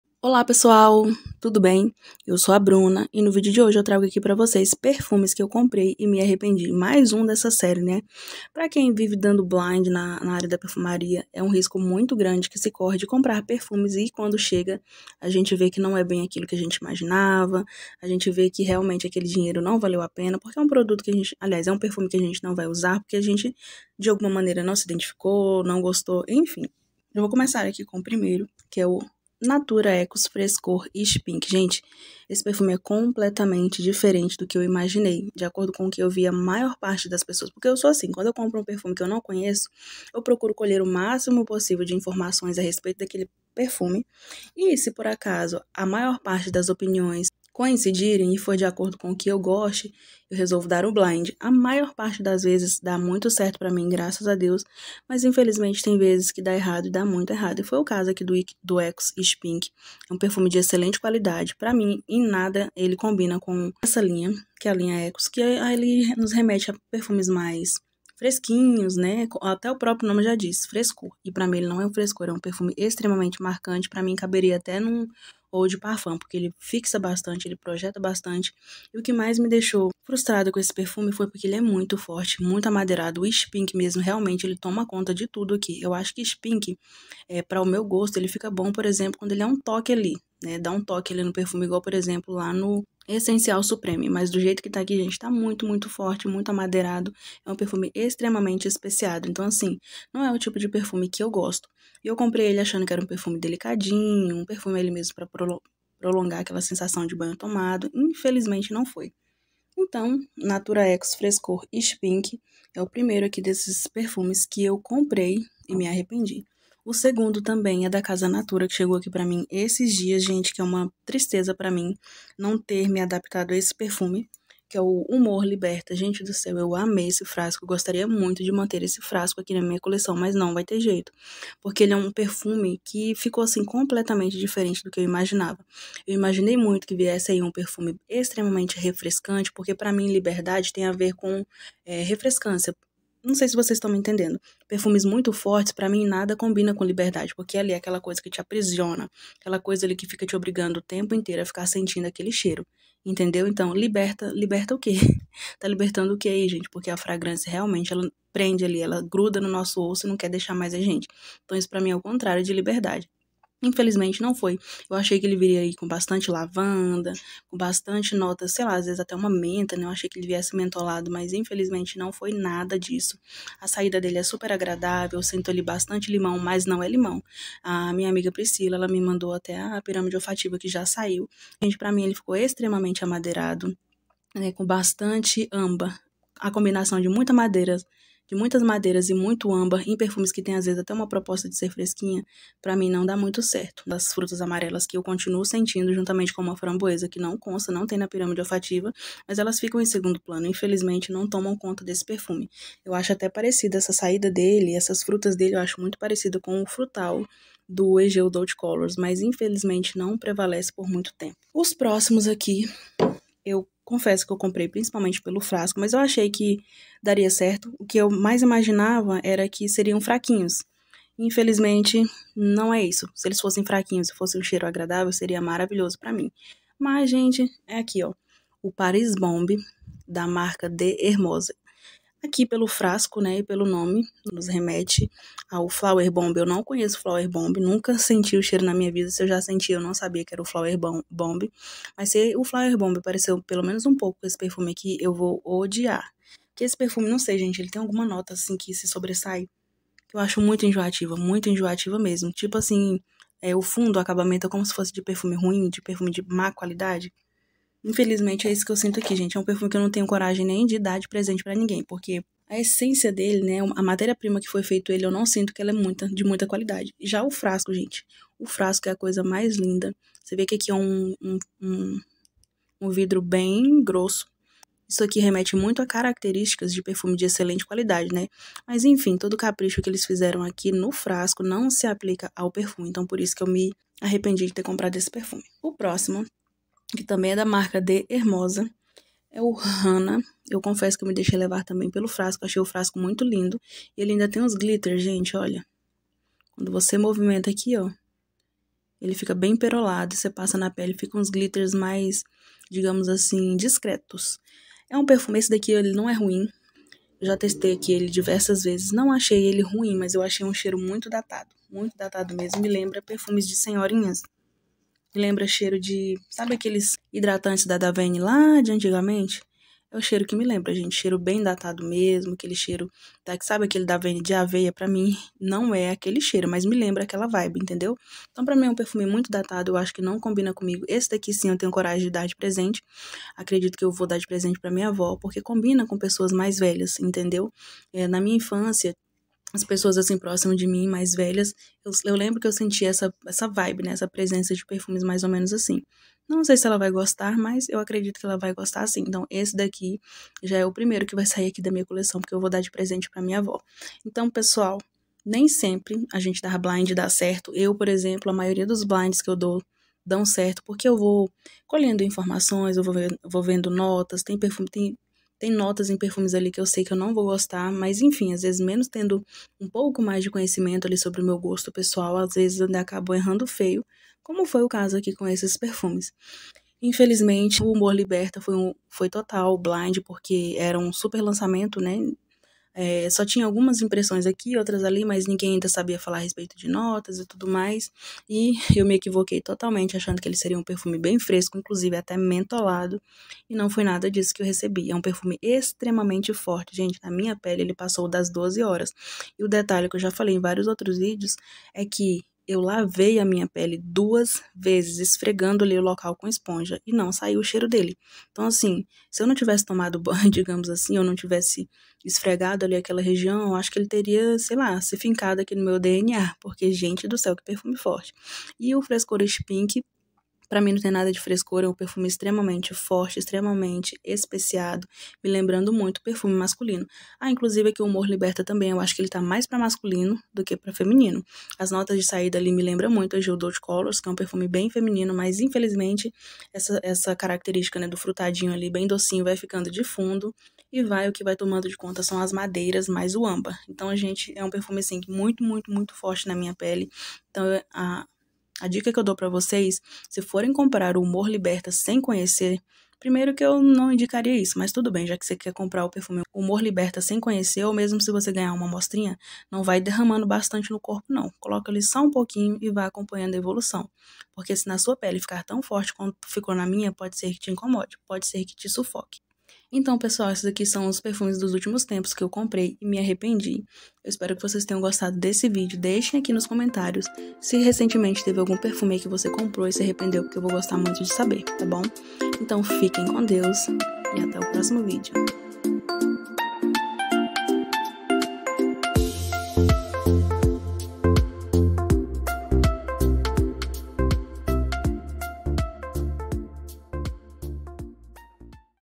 Perfumes que COMPREI e ME ARREPENDI🤦🏻‍♀️
Contei pra vcs os últimos perfumes que comprei e me arrependi 🤦🏻‍♀️